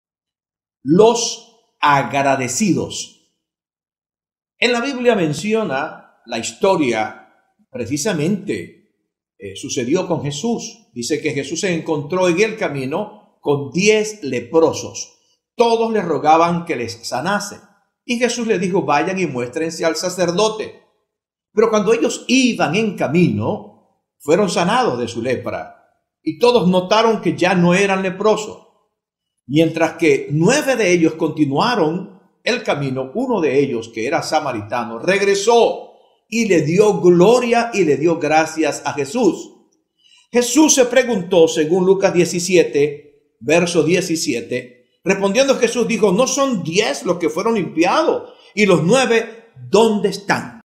PASTOR ISIDRO RODRÍGUEZ #117 | LOS AGRADECIDOS | MICRO HNO PUERTAS | LUZ TV
¨Respondiendo Jesús, dijo: ¿No son diez los que fueron limpiados? Y los nueve, ¿dónde están?¨
Lucas 17:17